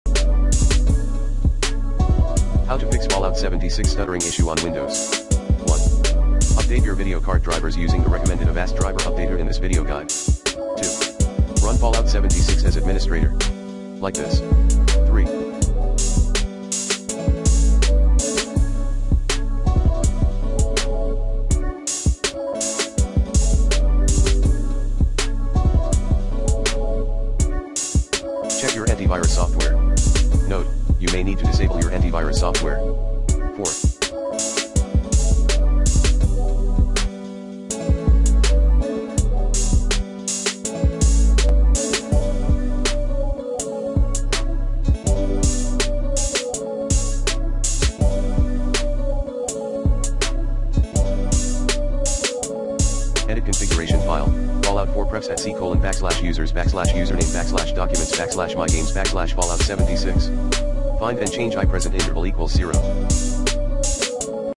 Fix Fallout 76 Stuttering issue on Windows
Here's how to Fix Fallout 76 Stuttering issue on Windows. Run Avast Driver Updater@ to keep your device drivers up-to-date, easily & effectively.